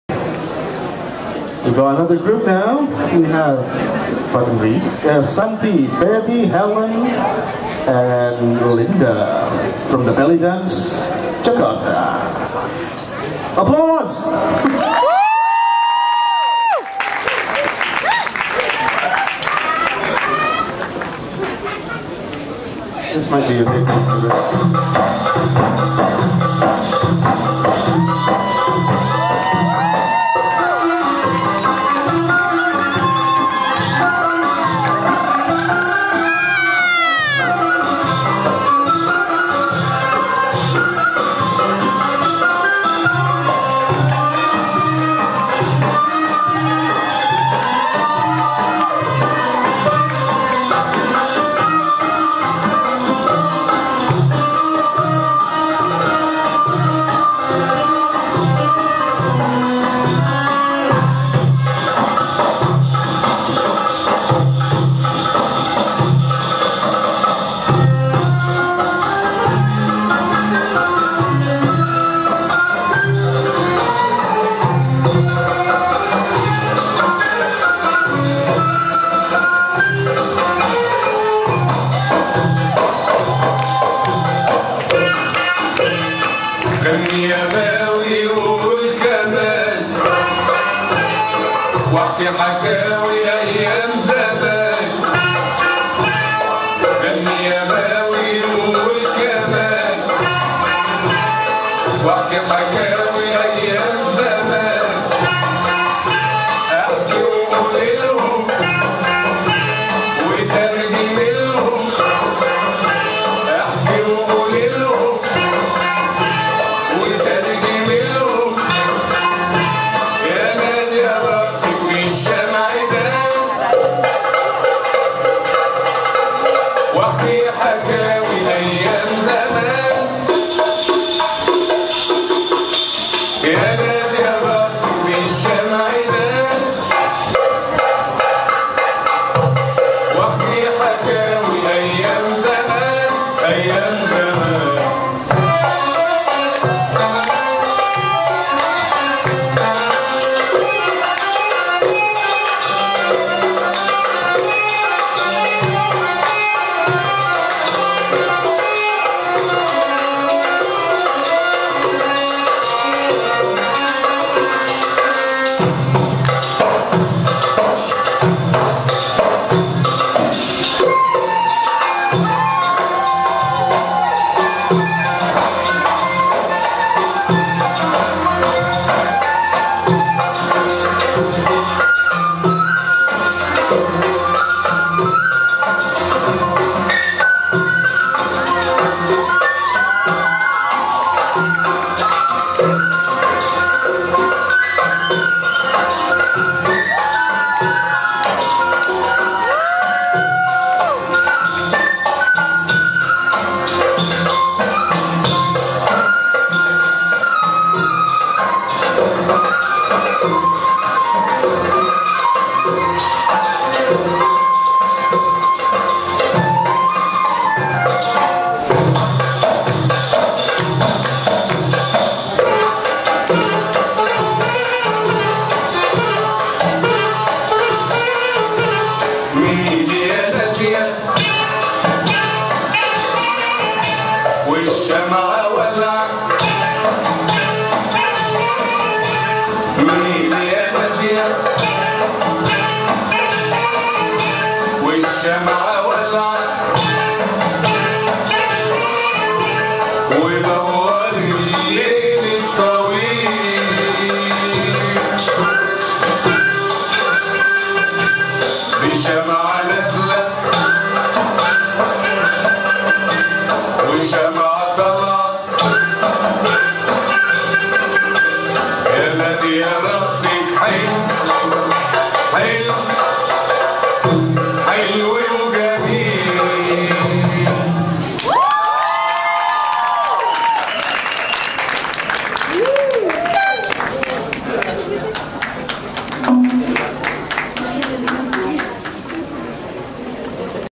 Bellydance Jakarta - Shamadan Performance Feb 2010
Bellydance Jakarta - Indonesia's premier Middle Eastern dance center
Shamadan performance by The Bellydance Jakarta Dancers
Choreography by Mehdat Fahmy, adapted by Christine Yaven (Modern shamadan choreography)
Dancers: Santi, Febi, Helen & Lynda